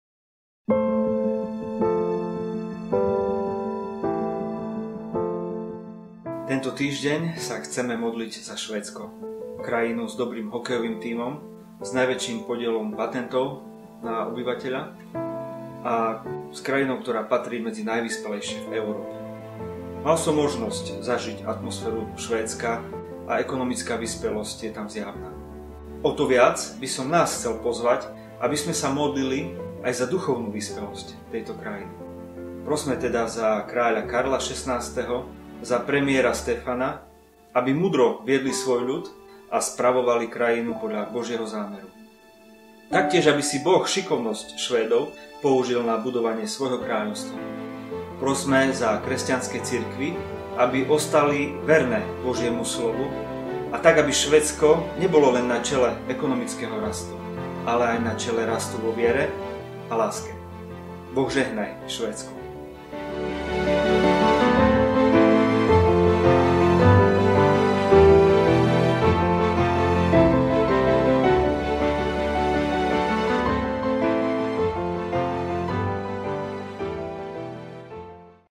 26. Peter Štupák - Švédsko
Tento týždeň sa modlíme za Švédsko. Pozývame vás k modlitbám za túto krajinu, ktorá potrebuje tiež naše požehnanie.